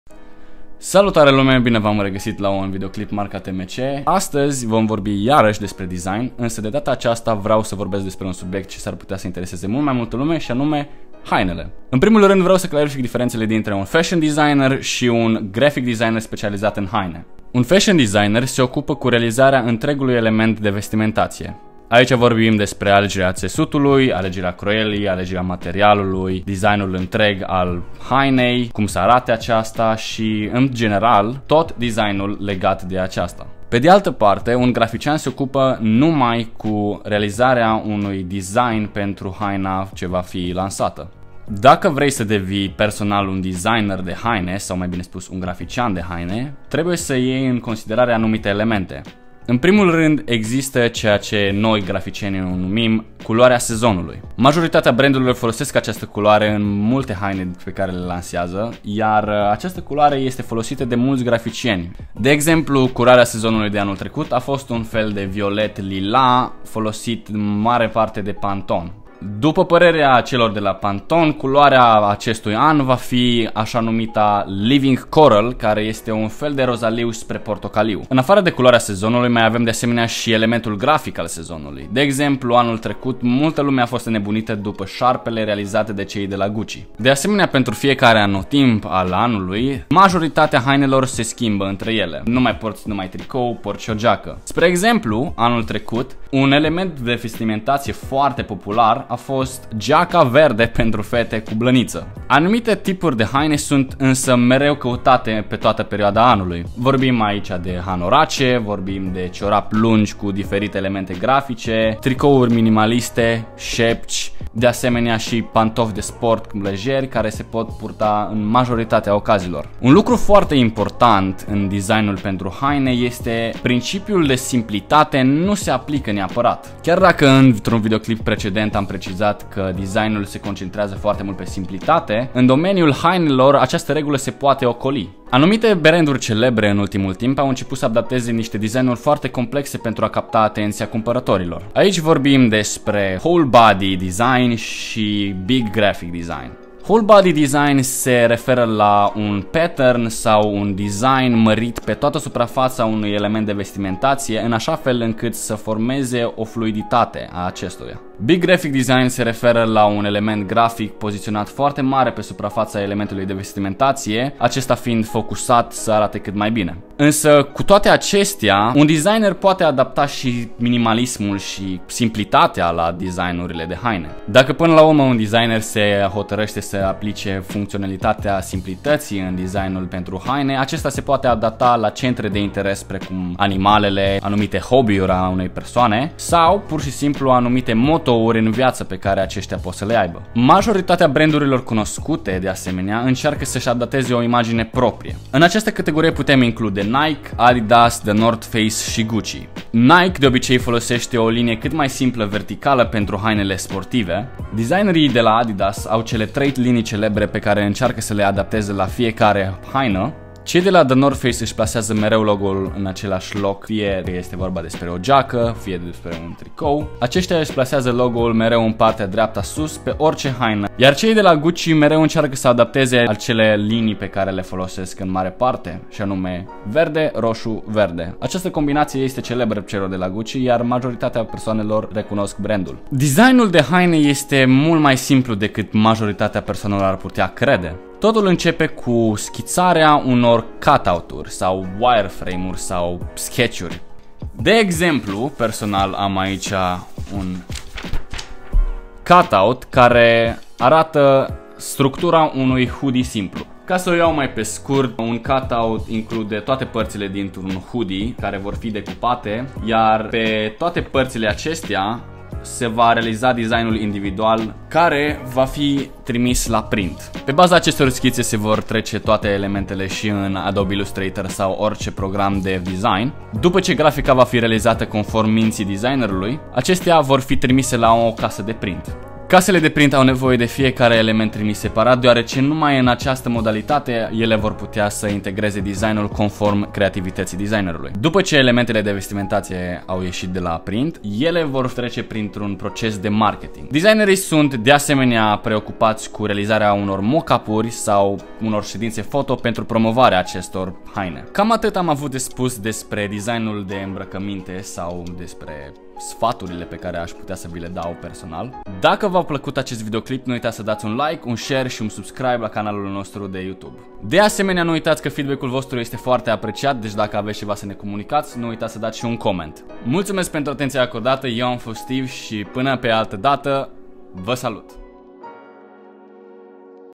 TE MIRI CE TE INVATAM DESPRE DESIGN - Grafica pentru Haine
In acest videoclip, Steve va impartaseste cateva informatii despre design-ul de haine si ce include acesta, dezvaluind anumit tehnici pentru a realiza o grafica atractiva.

Personal Instagram: @themilovan
Work Instagram: @stevan.designs

Asteptam propuneri in sectiunea de comentarii.